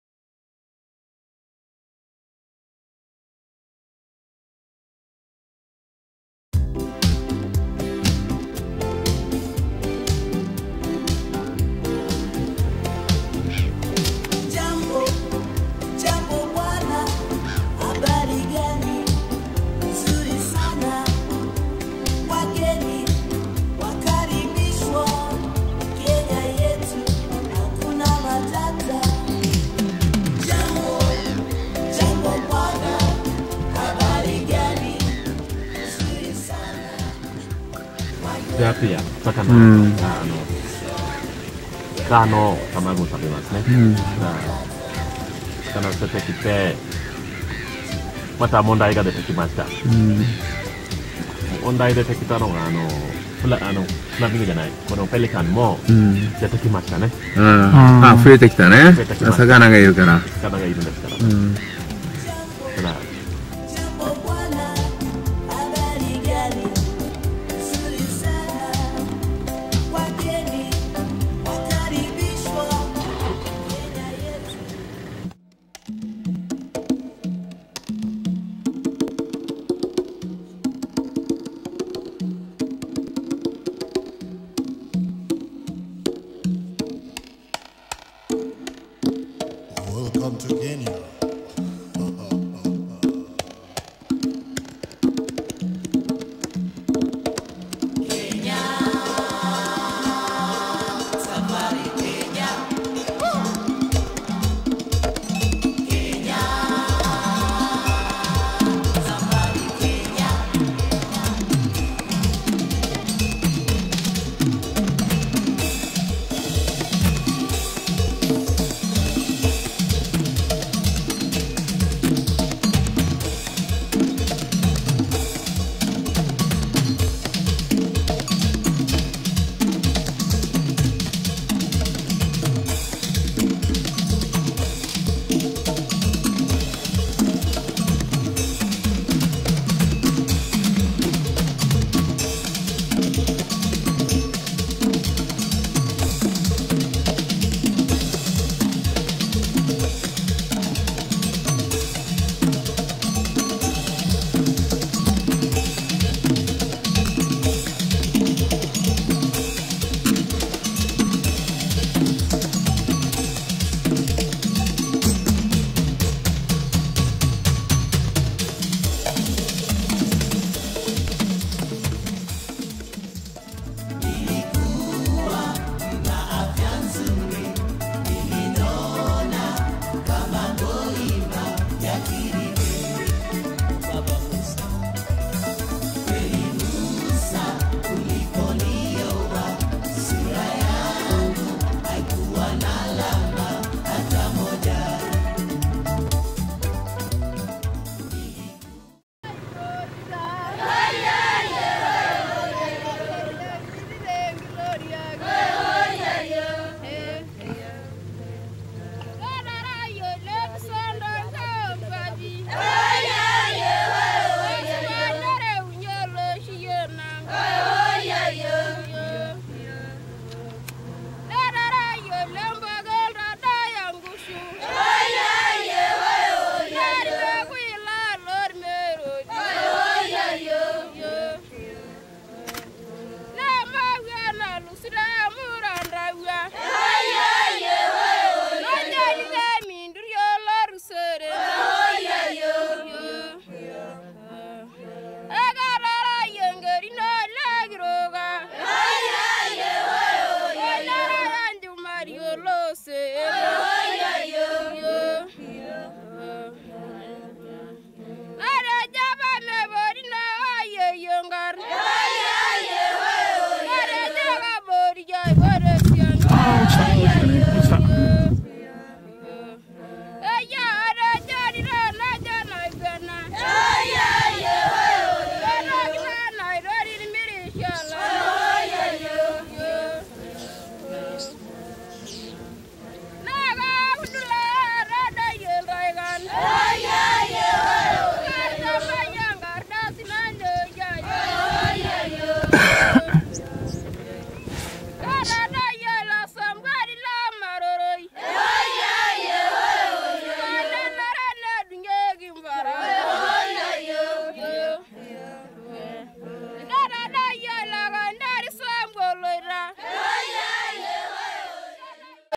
090304ナクル
アフリカにサファリにいきました。
サファリ初日はナクルからマサイマラへ。